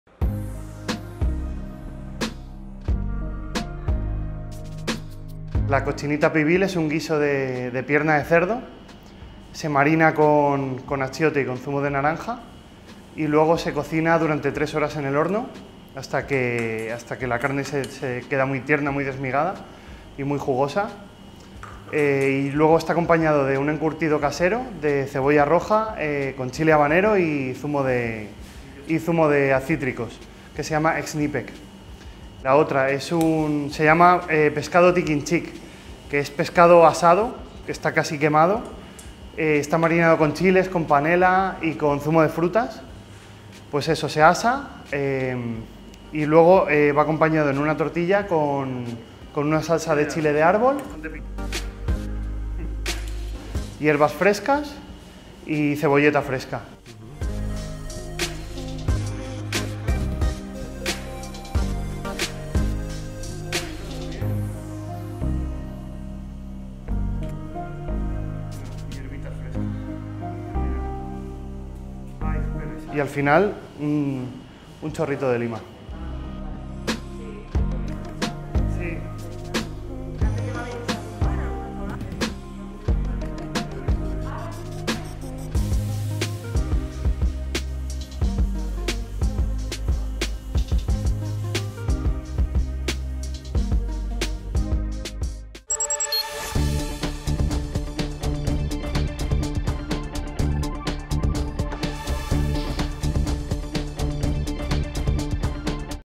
Descubre cómo cocinar estos nuevos tacos mexicanos de moda
Julián Barros, socio fundador de Mawey Taco Bar adelanta las recetas de cocina mexicana en forma de tacos que se pueden degustar ya en Mawey Taco Bar.